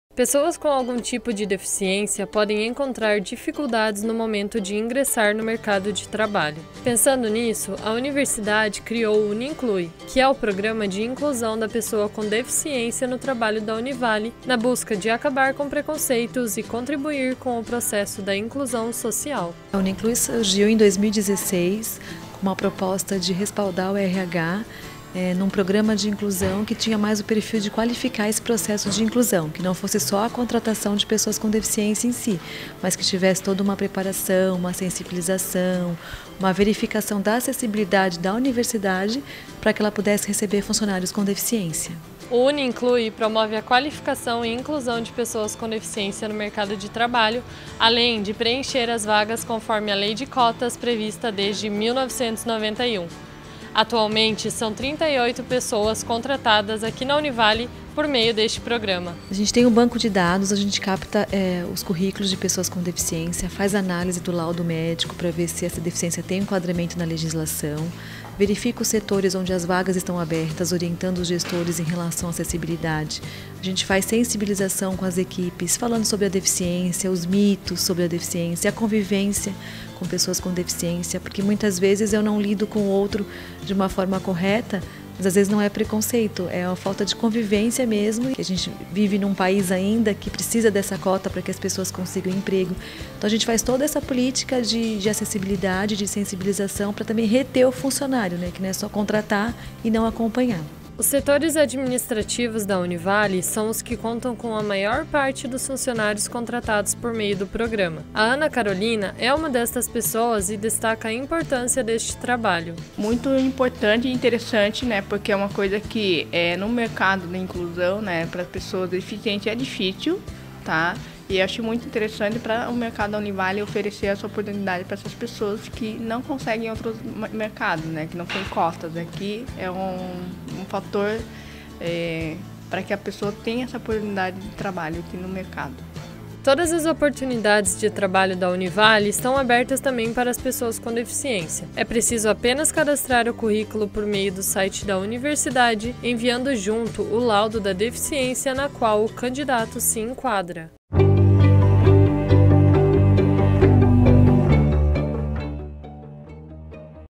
Programa de Inclusão da Pessoa com Deficiência no Trabalho da Univali
O Programa de Inclusão da Pessoa com Deficiência no Trabalho da Univali – Uninclui, promove a inclusão e qualificação de pessoas com deficiência no mercado trabalho. O programa também se relaciona com atores da sociedade civil e instituições atuantes no segmento, buscando firmar parcerias.

Desde sua criação, em 2016, o Uninclui identifica postos de trabalhos e possíveis candidatos para preenchê-los conforme a Lei de Cotas, art.93 da Lei nº 8.213/91.

O Uninclui contribui para a inclusão da pessoa com deficiência e cumpre, a médio e longo prazo, a cota da Universidade como prevê a legislação. Por meio de um banco de dados, o programa seleciona e respalda os candidatos, orientando para o enquadramento, assistência social e encaminhamento.